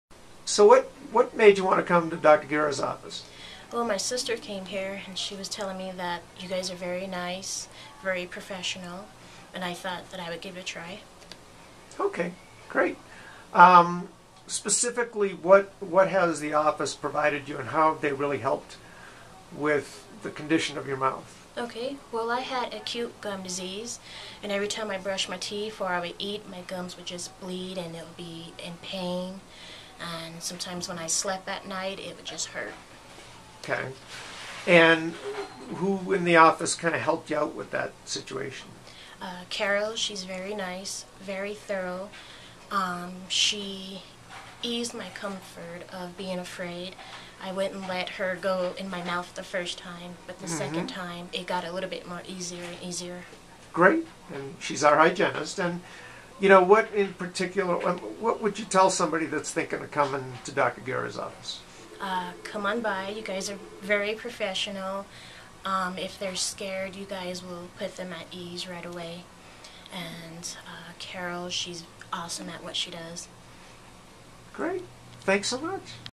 Cosmetic Dentistry Dr Fred Guerra Colorado Springs, CO
- Colorado Springs, CO cosmetic dentist Dr. Fred Guerra, DDS provides cosmetic, general and family dentistry to his patients of Colorado Springs, Colorado. From routine teeth cleanings to complete cosmetic dental restorations, this Colorado Springs cosmetic dentist is well skilled and experienced in many fields of dentistry. or call our office at for more information or to schedule an appointment to see Dr. Guerra